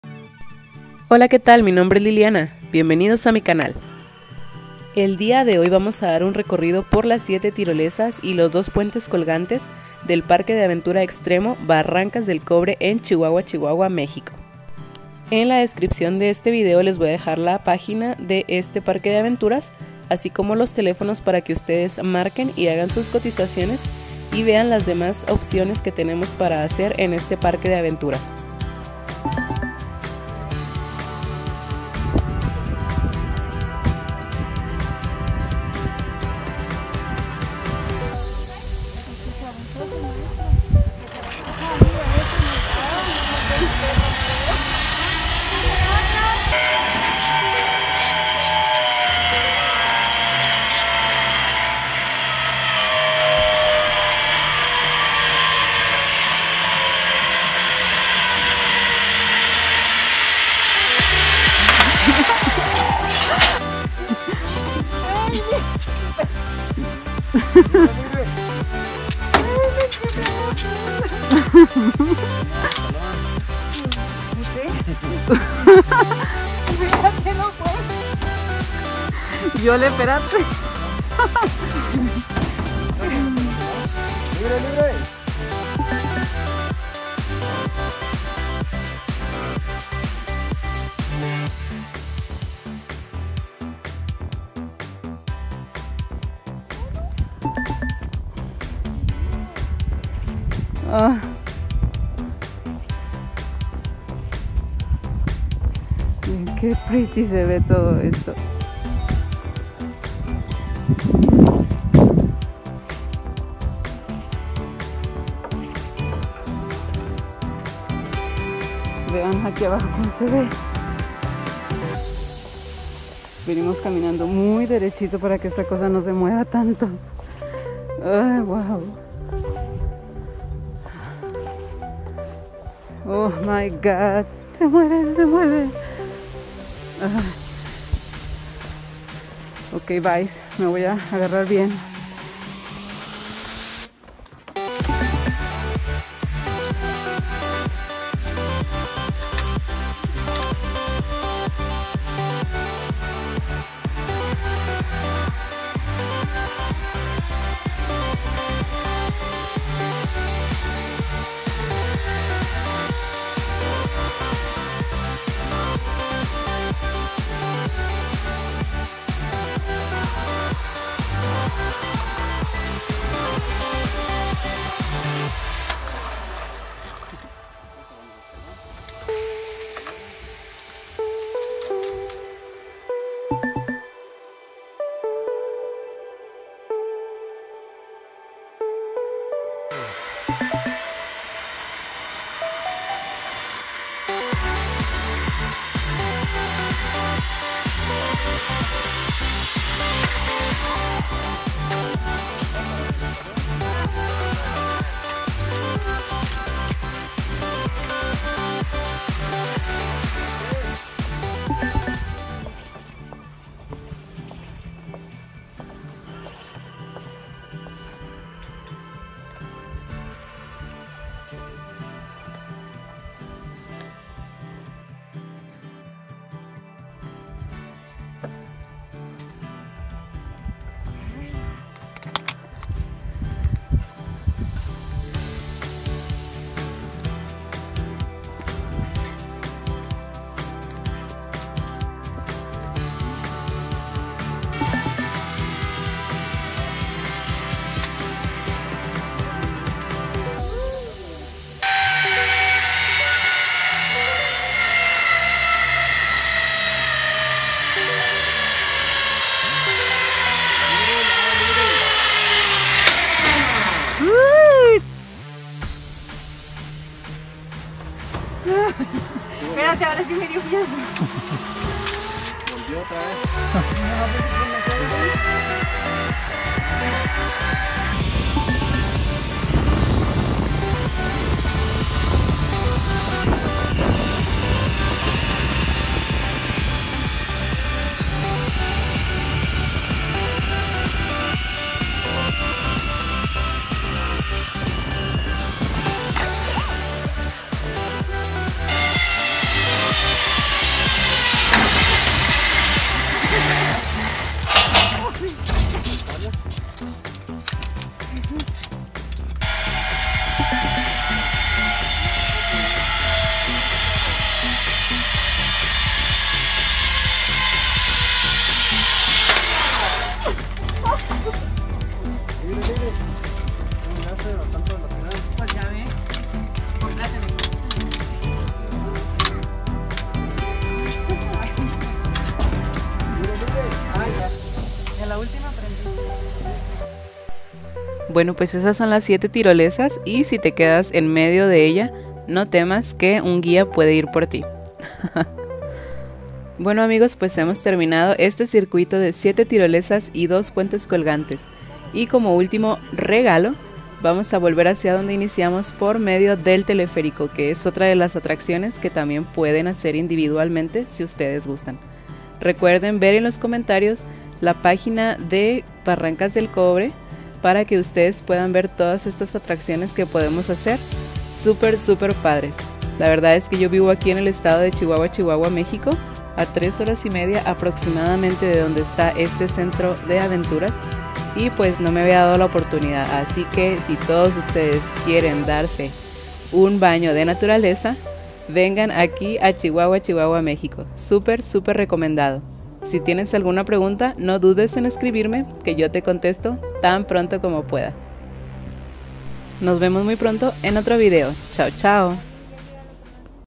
TIROLESAS BARRANCAS DEL COBRE SIERRA TARAHUMARA CHIHUAHUA ⛰
Hola amigos, en esta ocasión les dejo este video de mi ultimo viaje a la sierra de chihuahua para cerrar el año 2020. Ya había ido, pero no me había dado la oportunidad de vivir esta experiencia que por cierto me encanto, es una experiencia super padre para cualquiera.

Esta es la pagina del Parque de Aventura Barrancas del cobre

Actividades para hacer:
* Caminata Ferrata
* Circuito 7 tirolesas + 2 puentes colgantes
* Teleferico
* Zip Raider
* Ciclismo
* Bosque Aereo

Y para los mas pequeños:
* Rocodromo
* Euro Bongi
* Golfito

Les dejo tambien los links de las otras actividades que hicimos en la sierra:

Super super recomendado... si tienes alguna pregunta no dudes en escribirme que yo te respondo tan pronto como pueda!!

Nos vemos muy pronto en otro video. ciao ciao!!!